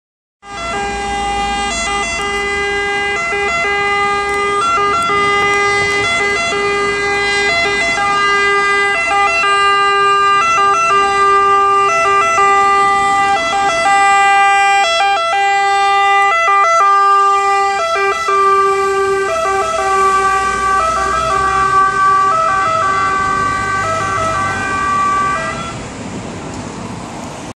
ambulanza volontari pubblica assistenza cremona in emergenza
Descrizione:
un' ambulanza su Renault master arriva in emergenza al pronto soccorso dell' ospedale di cremona con dispositivi luminosi e sonori in funzione.
english:
The ambulance of "volontari pubblica assistenza" cremona arrives at the hospital in emergency